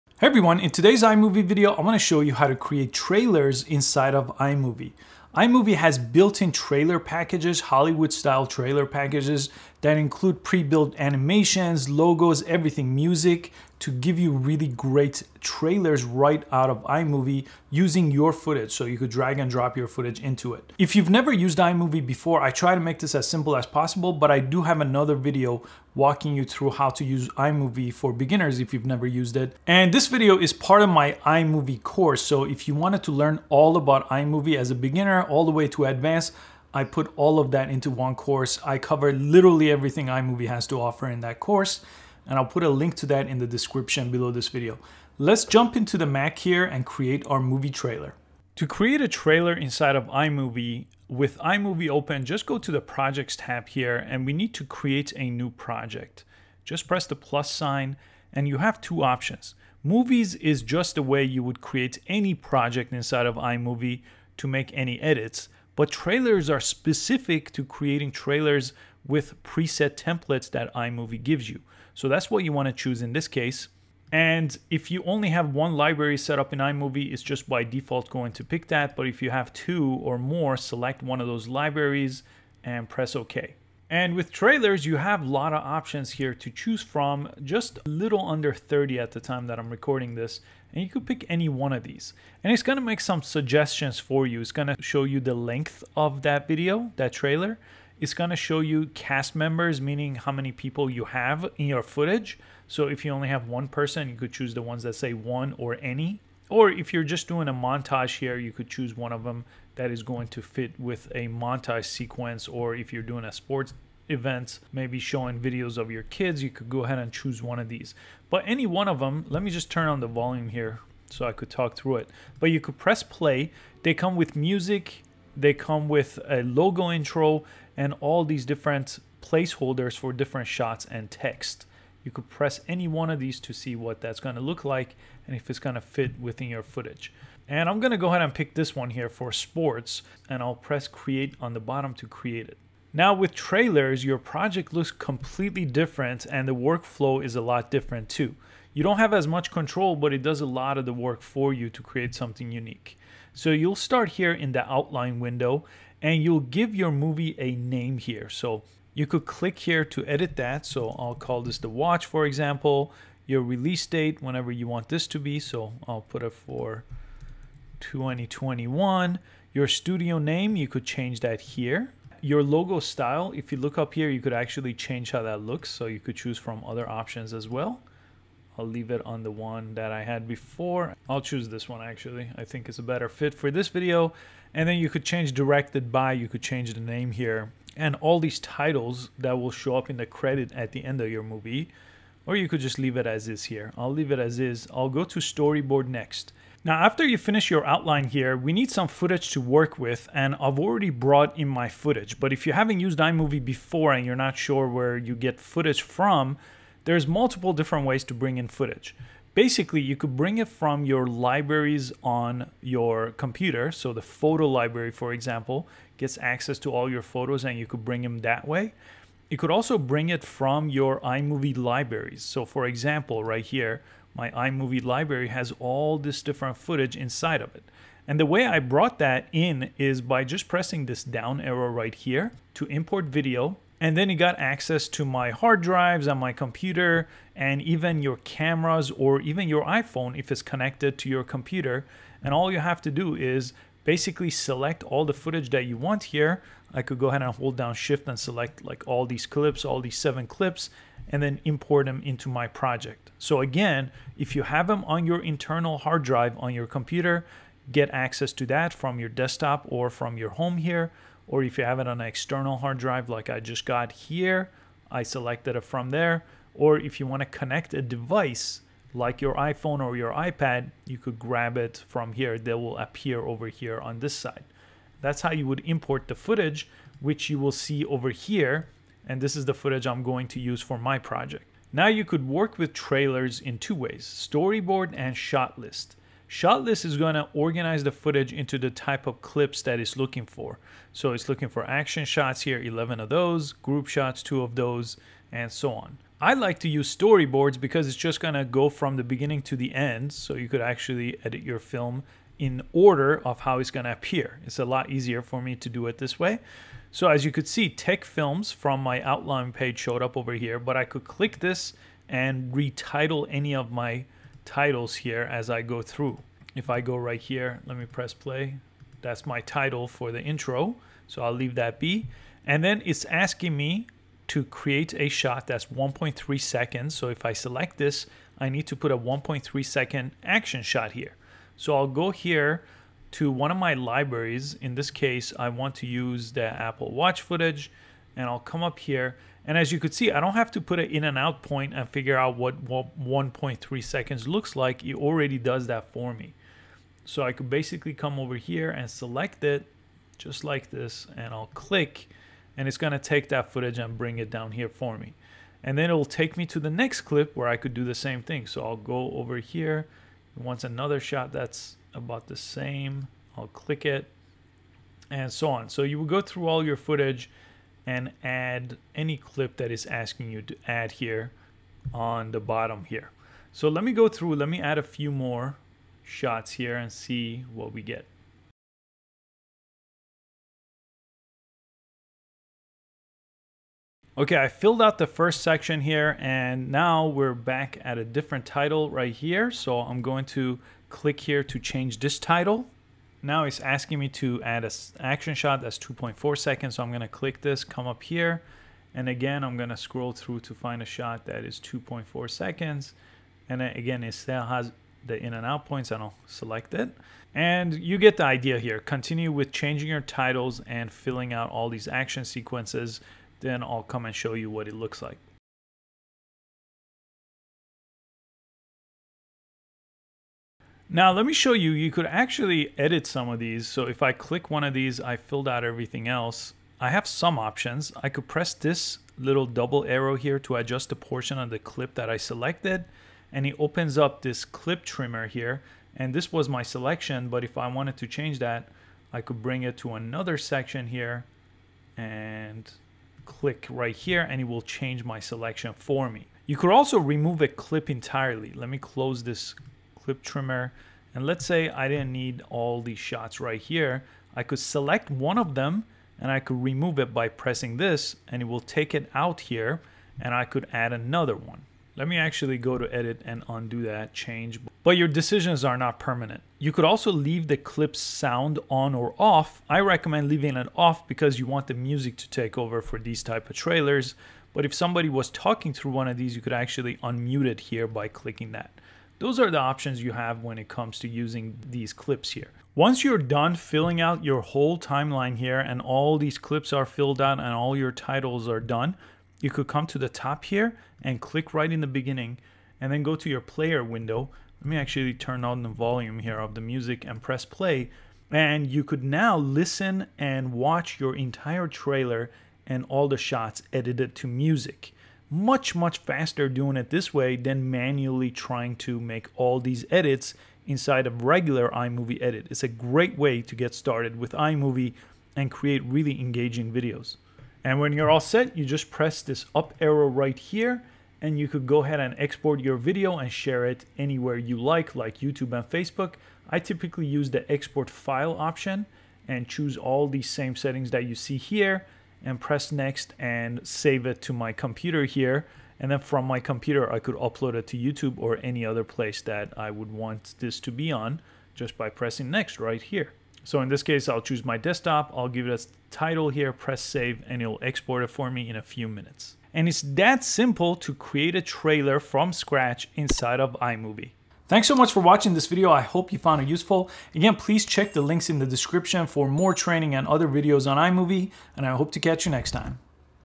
How to Make iMovie Trailer
iMovie is the default editing app that comes with every Mac.

And with iMovie, you get Hollywood style trailers.  These trailer templates are easy to use and help you get started with iMovie right away.
If you haven't used iMovie before, watch this video.

And if you want a more in-depth iMovie training, check out iMovie Beginner to Advance: The ultimate iMovie Course

To create a trailer in iMovie, simply press create new and choose the trailer option.  You can watch a preview of any trailers you see before creating a project based on that trailer.
Once you have the project created, you can drop in video clips, change the text, and add your own voice or music.
Your projects are organized in outline and storyboard and shot list.  You can change all the things you see here like naming your film, adding credits, changing the shots in the storyboard, and so on.

Once you are done, you can share the video as a file or on YouTube or Facebook, or any other video platform.

We'll cover all this during this tutorial.